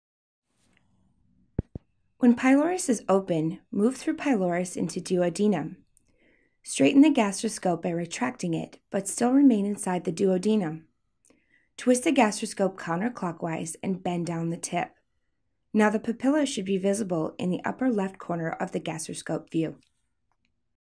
EndoSim Upper GI: Passage of pylorus
Using newly developed dynamics for a fully simulated, responsive scope, our upper GI exercises give the learner the possibility to train on scope handling, navigation to duodenum and back, and mucosal examination; all to maximize technical skills and minimize risks before the first meeting with the patient.